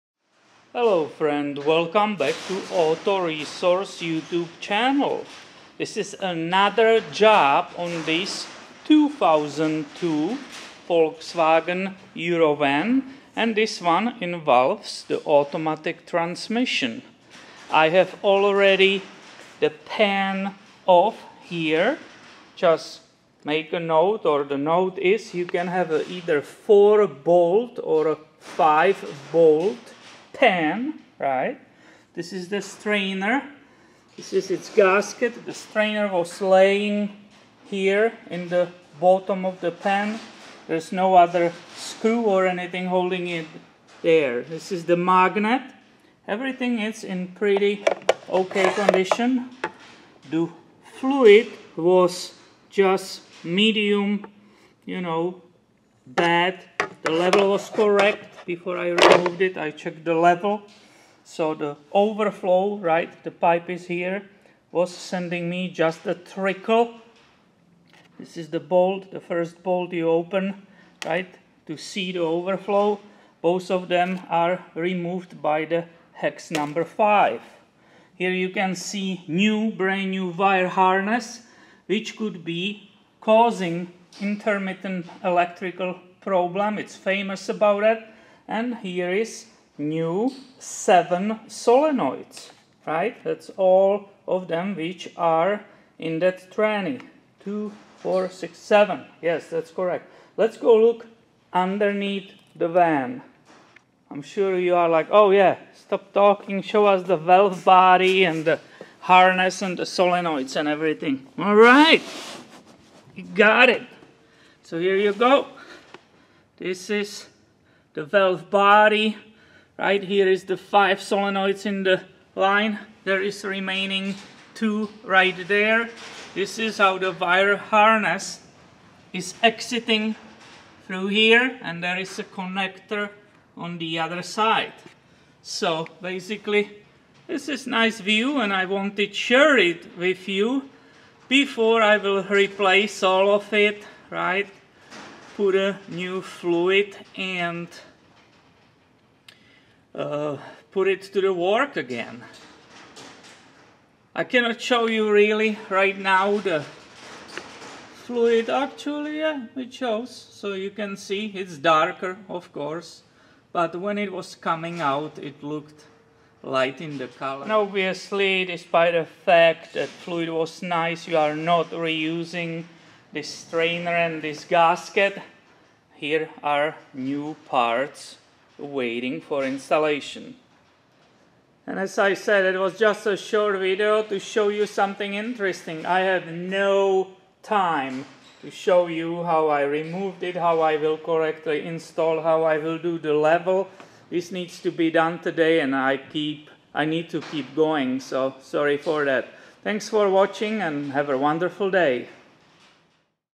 VW Automatic Transmission Solenoids, Wiring Harness, Strainer
Look inside of Volkswagen automatic transmission